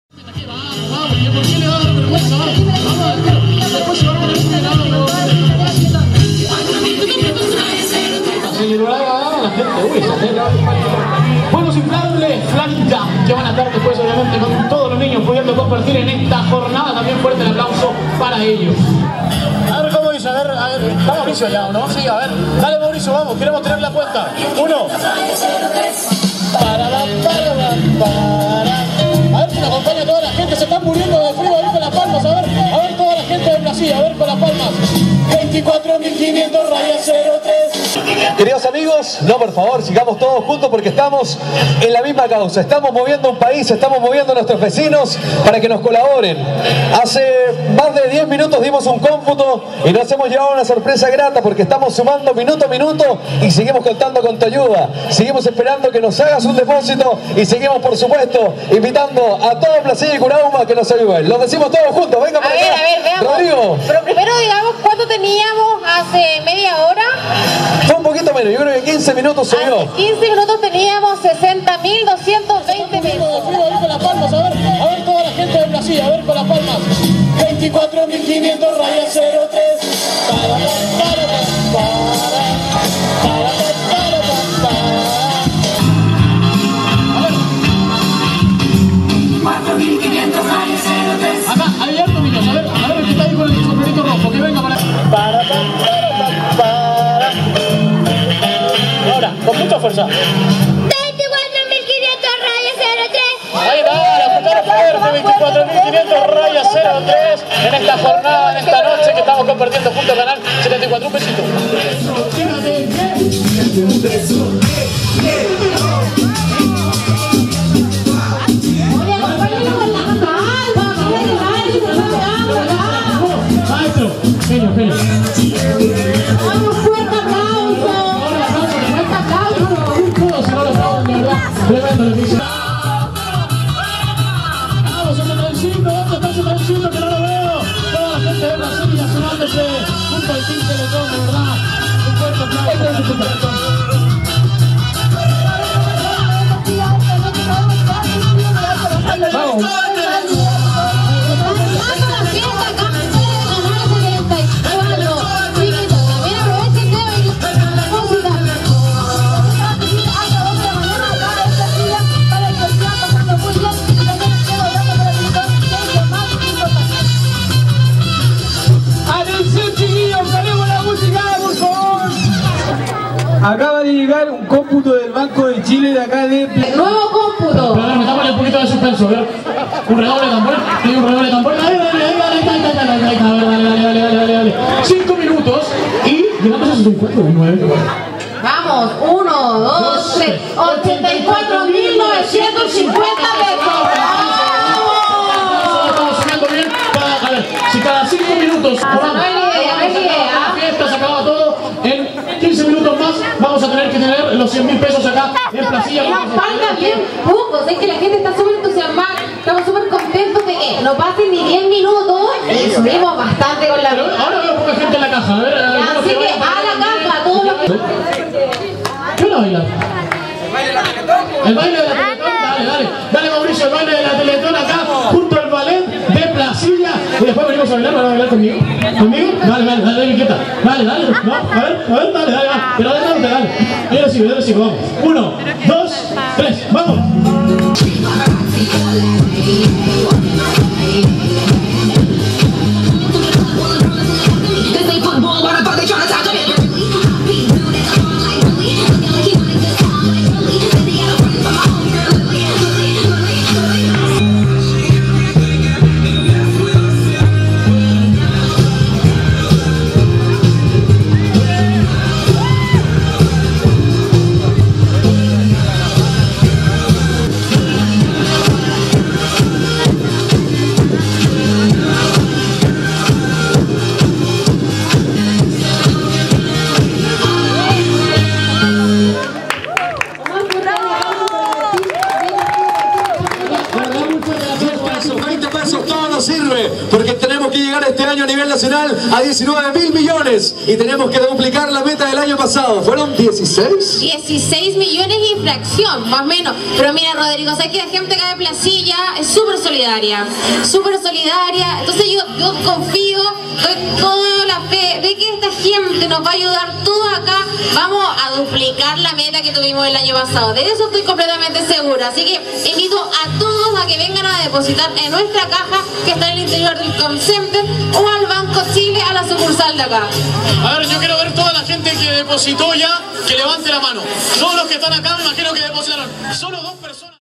Teleton 2011
Making of: The lokal channel of La Placilla, Chile, presents the Teleton charitity event 2011.